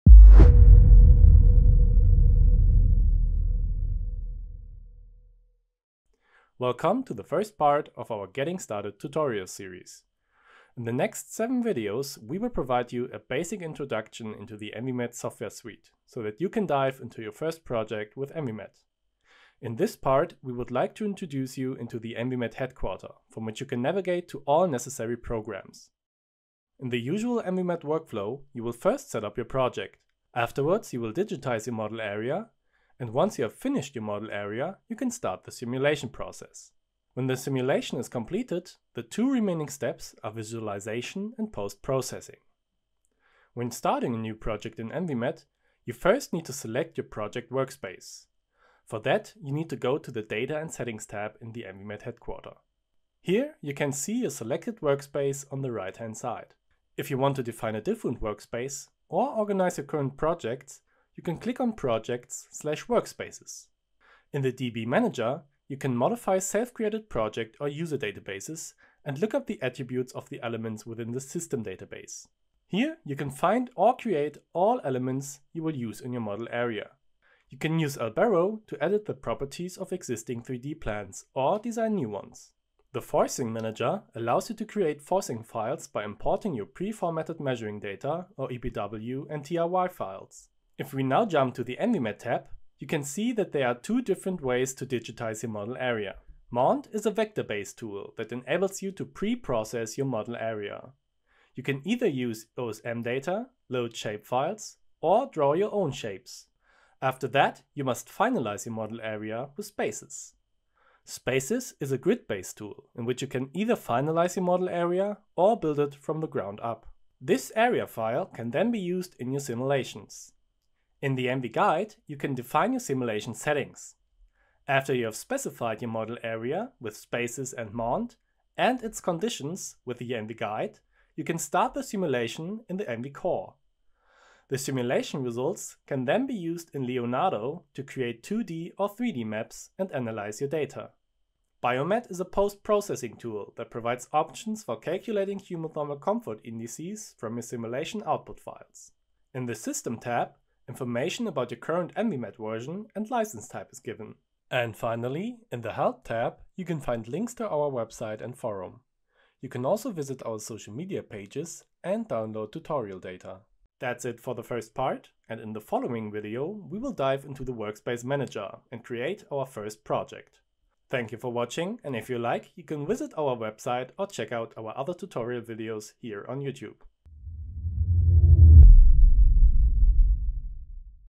Getting Started | Exploring the ENVI-met Workflow #1
Dive into the typical ENVI-met workflow. We'll tour the ENVI-met Headquarter, granting you access to all applications within the suite. As we proceed, we'll highlight the functions of these applications, offering a glimpse of ENVI-met's vast capabilities.